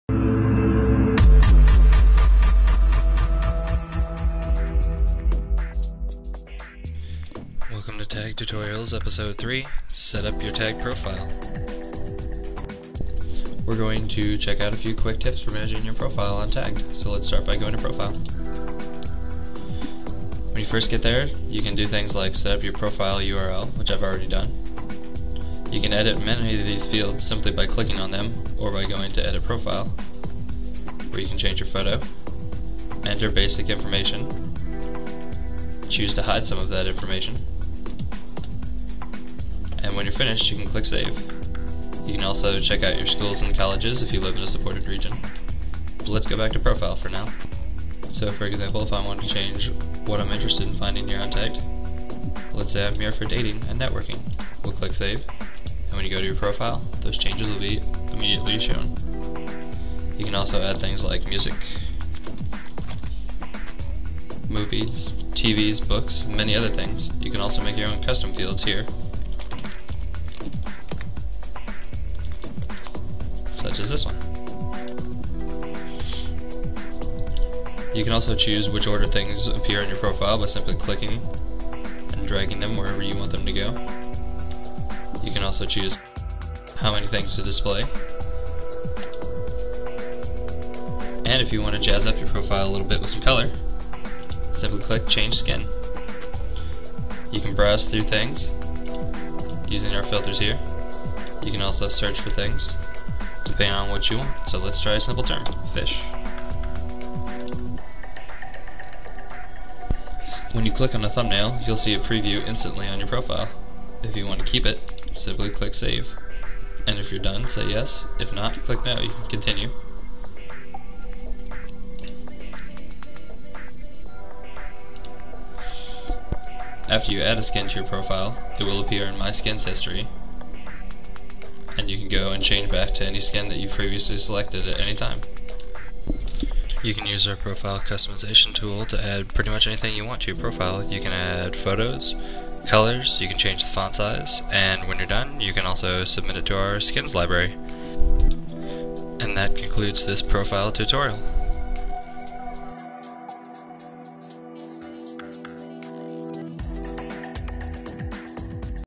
Setup Your Tagged Profile
Tagged tutorial, Episode 3: Setup Your Tagged Profile.  In this video, we'll show you how to setup your basic profile info and customize your page with a variety of skins- you can even make your own!